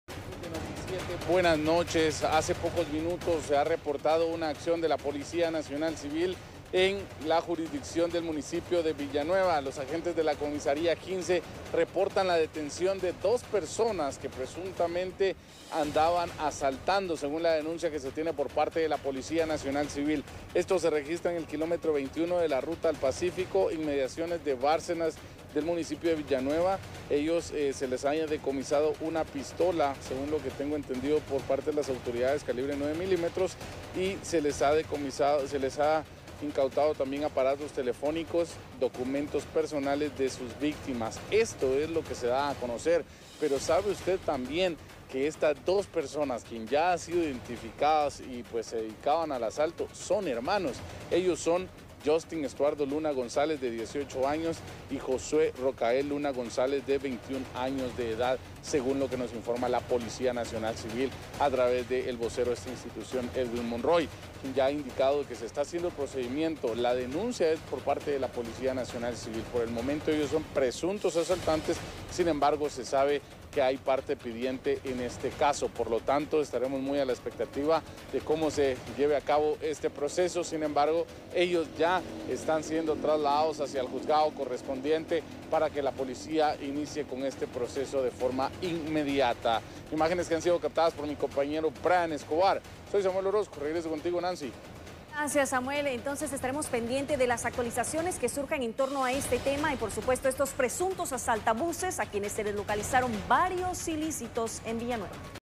PNC captura a dos "asaltabuses" en Villa Nueva
La aprehensión se registra sobre el kilómetro 21 de Ruta al Pacífico

Las fuerzas de seguridad lograron la captura de dos sujetos acusados de asaltar buses en el municipio de Villa Nueva.